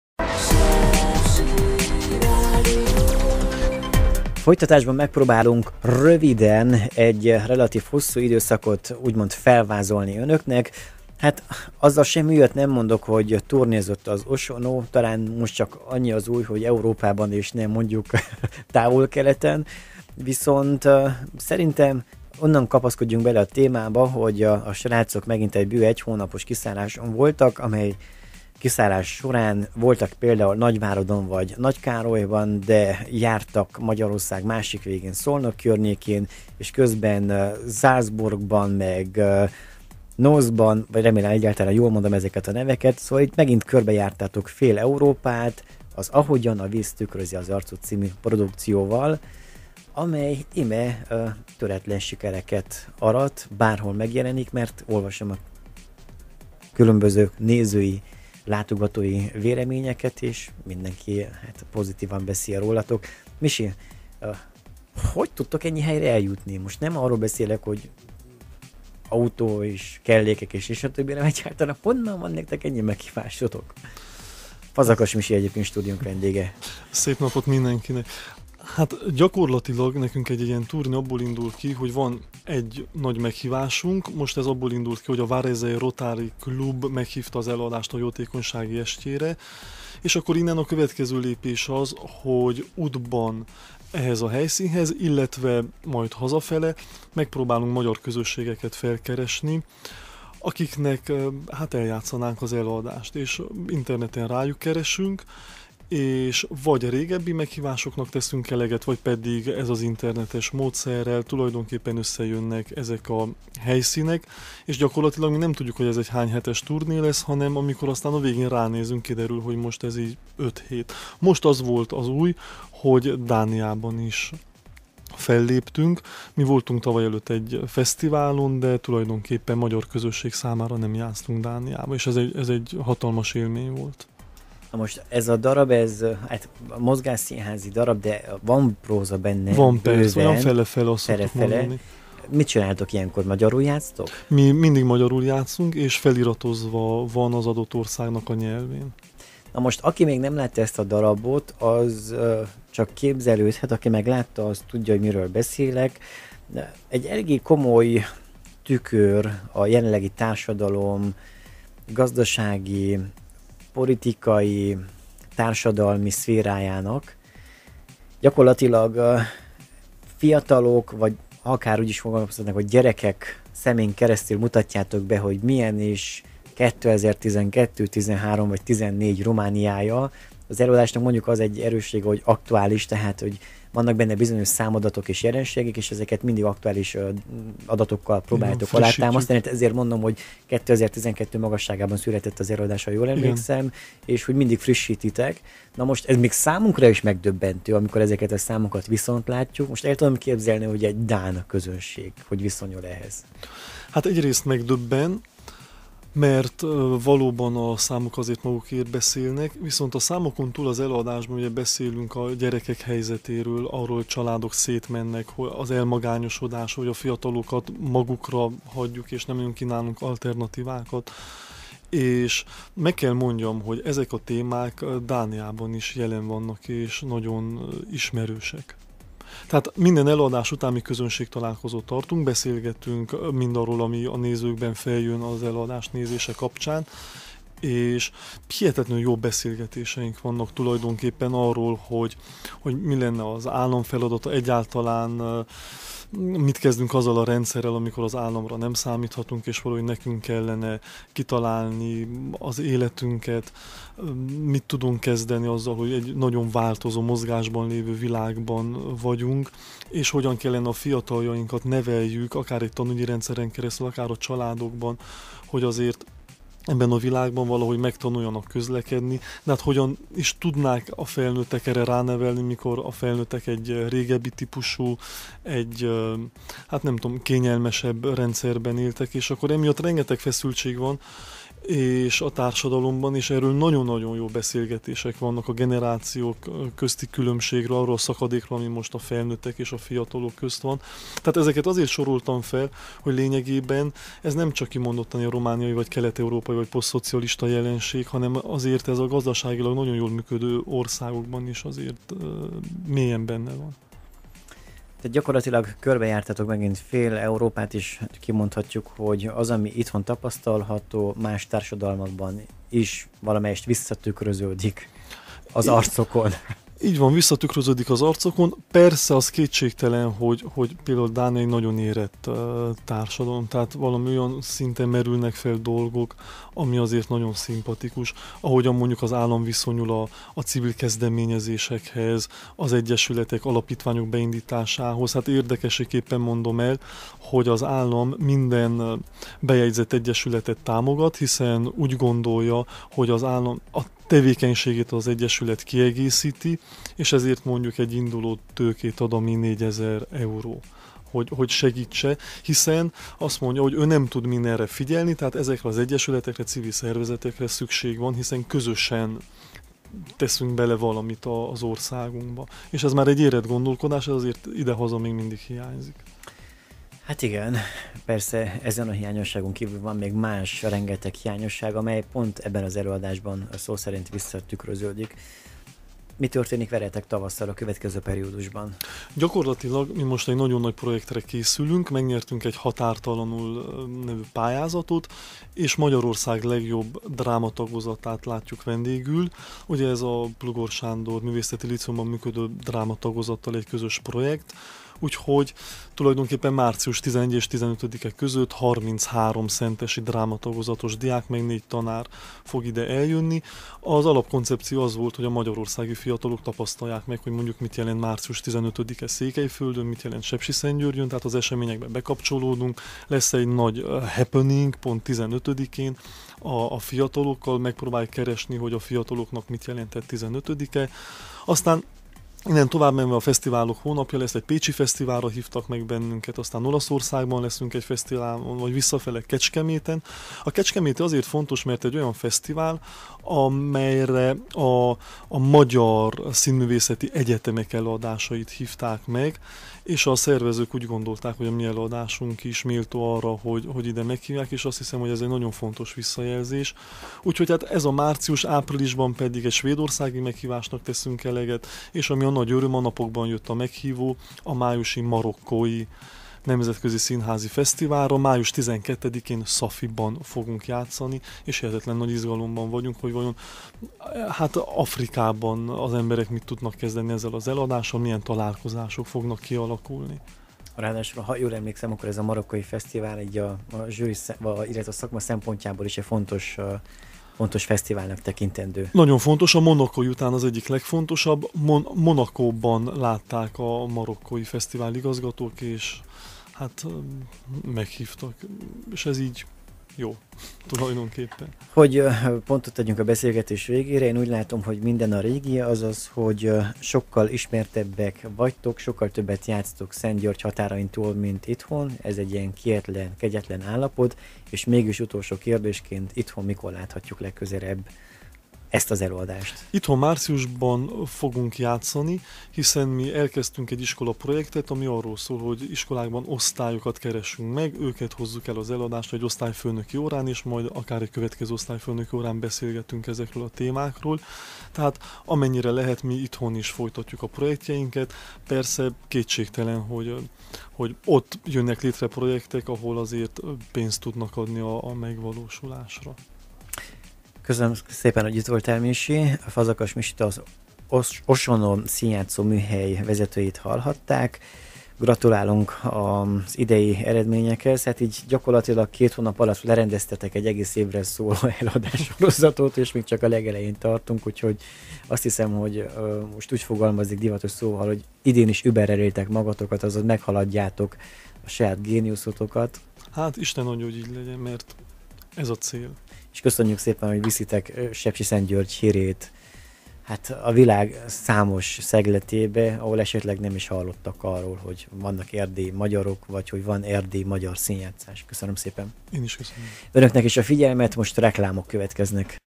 Osonó Színházműhely - Sepsi Rádió / Európai turnéval kezdi a 2014-et az Osonó
További információk és videók a weboldalon